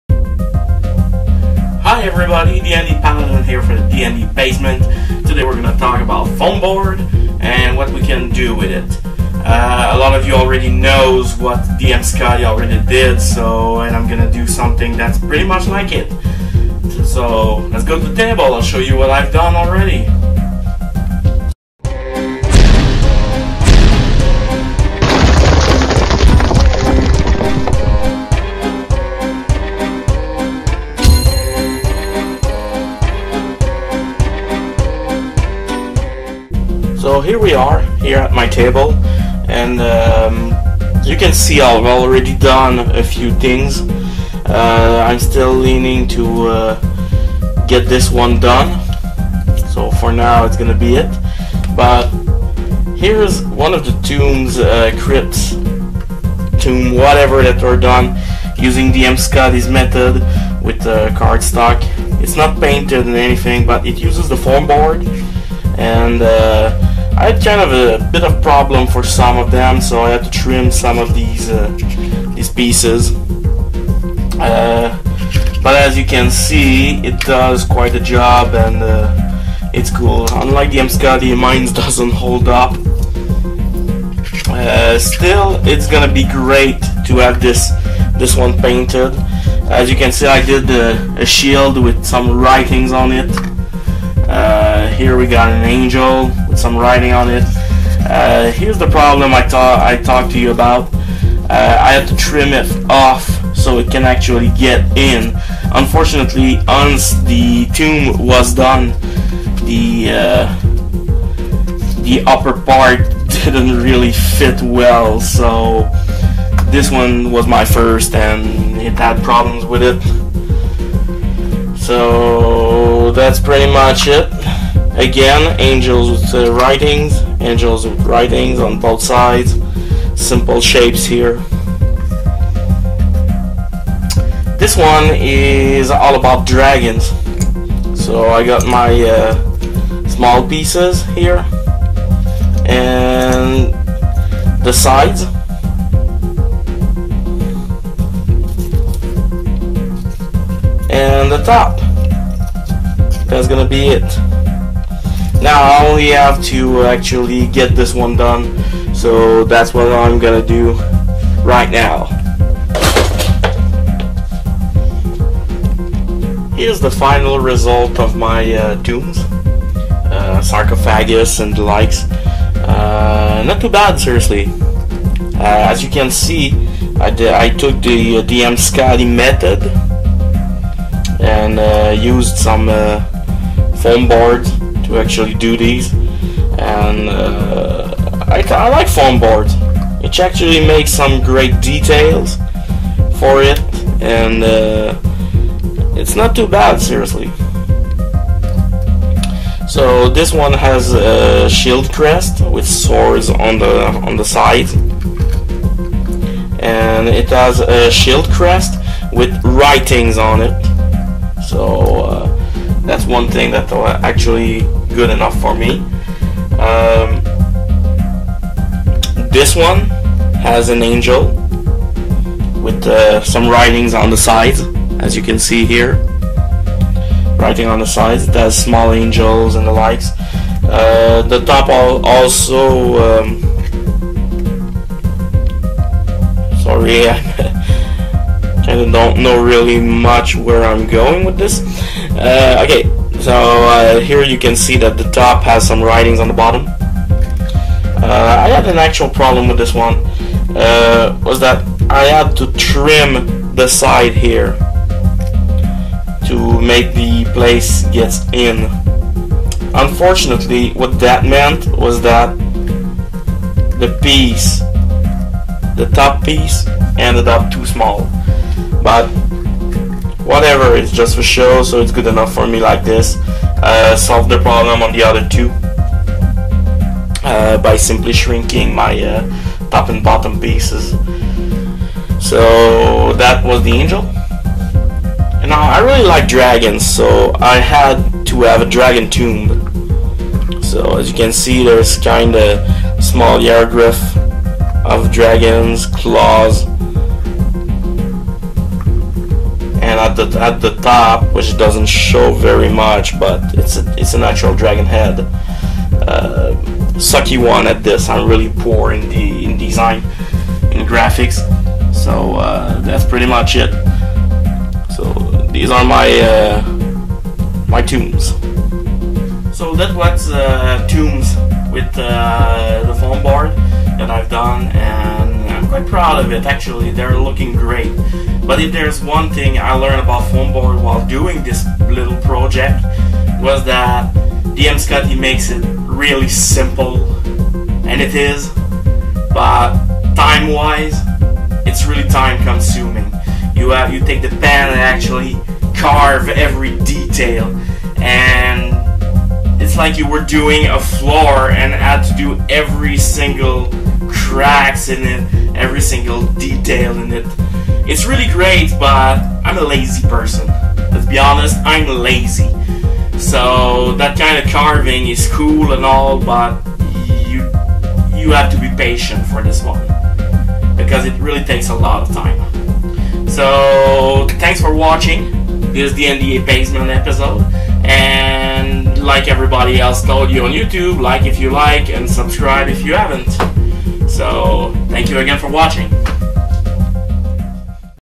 DnD Basement - Tombs
I'll show you my tombs made out of foamboard which i finally got my hands on. pretty much the same as DM scotty.